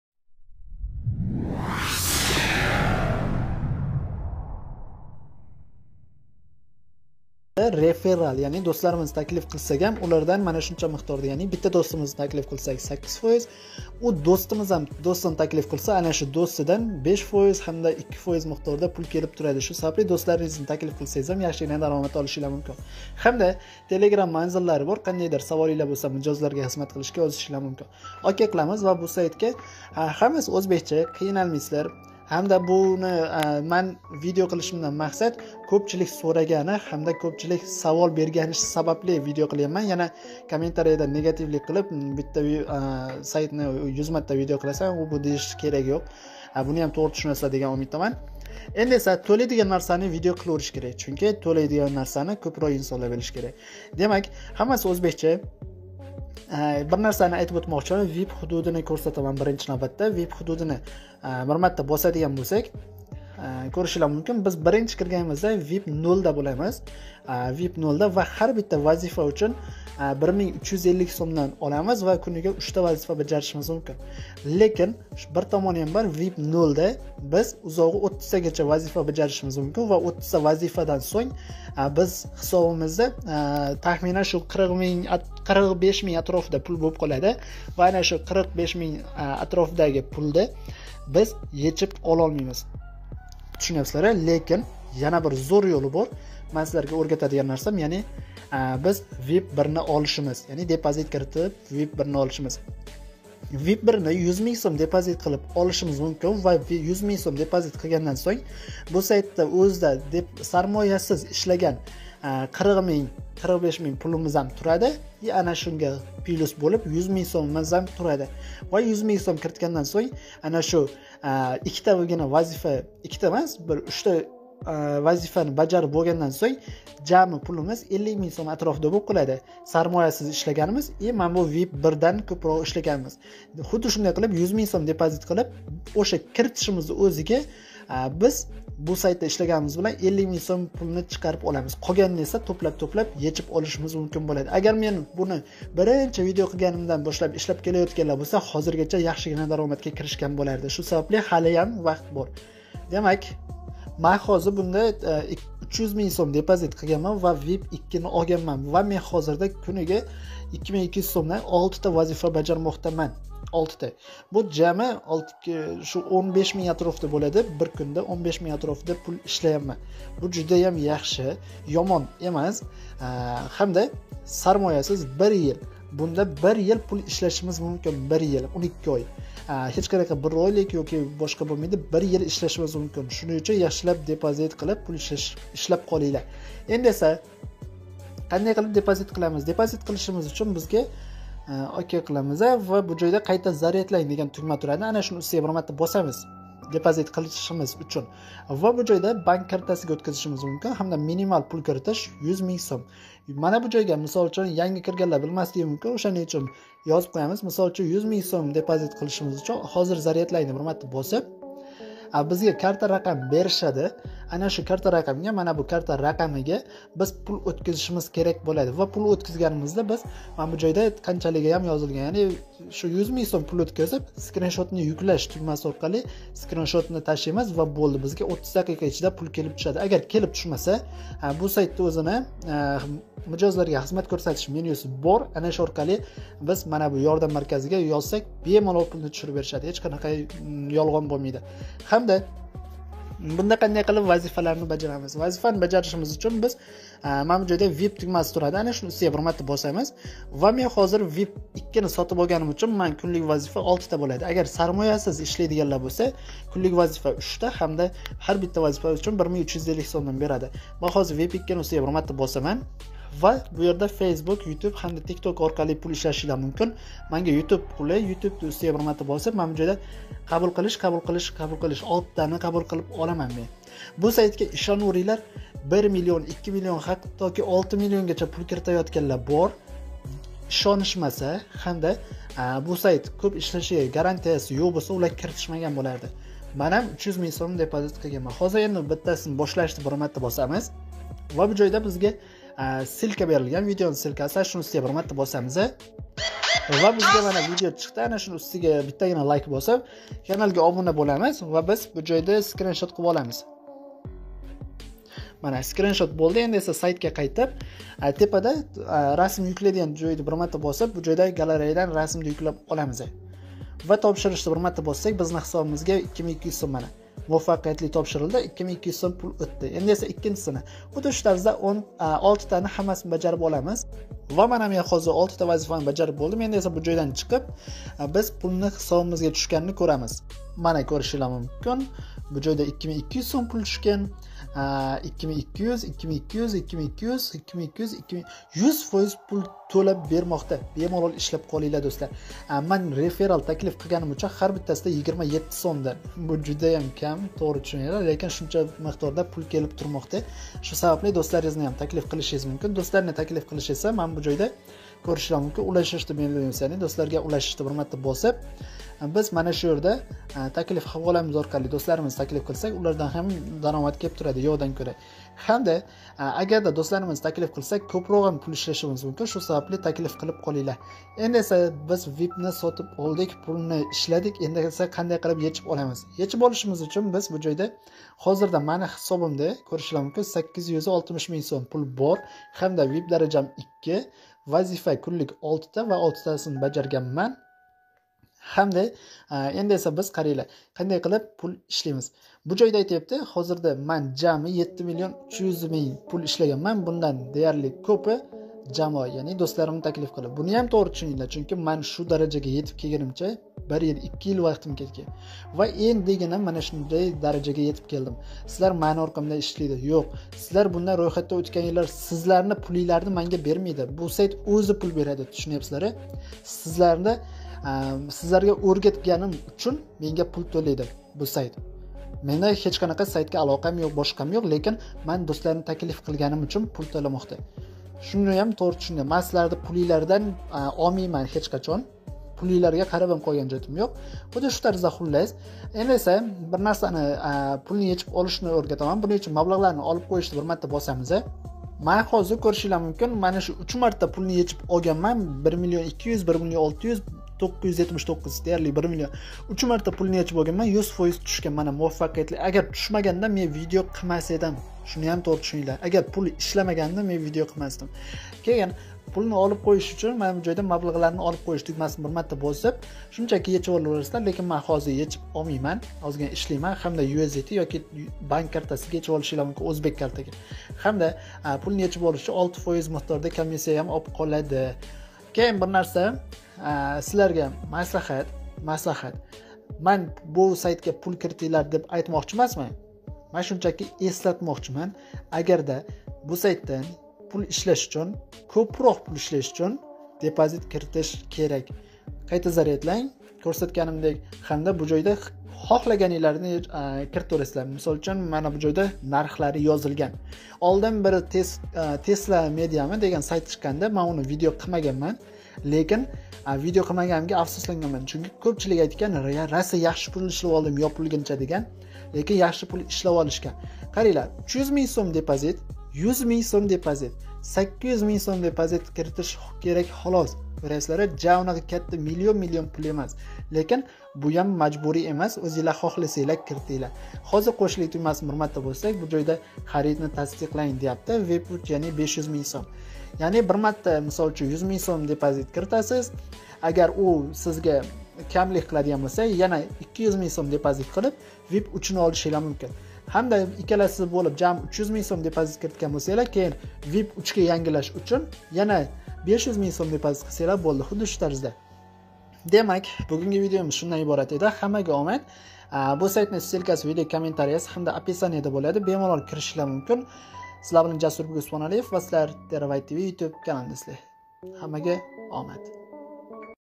Internettda Pul Ishlash Yŏllari 2022 |Sarmoyasiz Kuniga 5$ Dan Pul Ishlimiz | Pul Tŏlidigan Saytlar
2022 yilda internetda sarmoyasiz pul ishlashning yŏllarini birgalikda ŏrganamiz LEAH platformasi